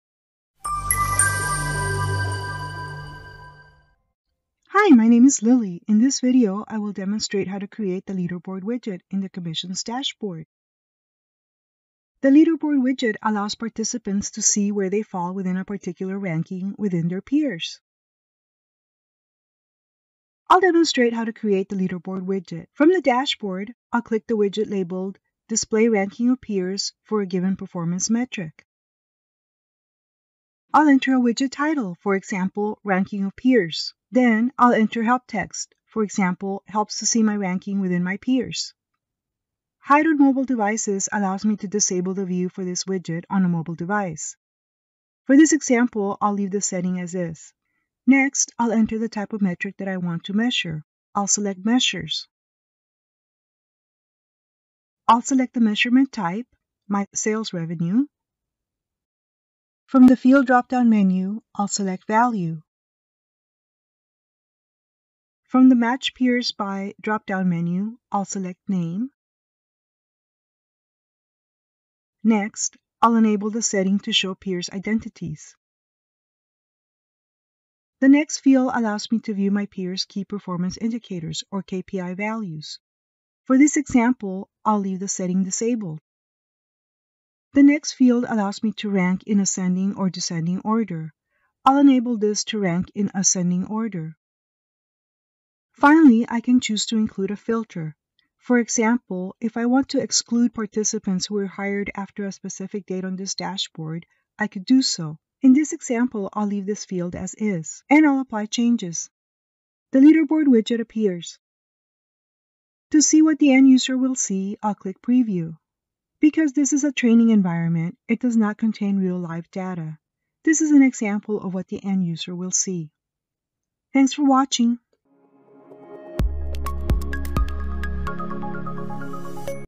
Dashboards: Leaderboard Widget: SAP SuccessFactors Incentive Management
This
 video demonstrates how to configure the Leaderboard Widget in SAP SuccessFactors Incentive Management  Dashboard.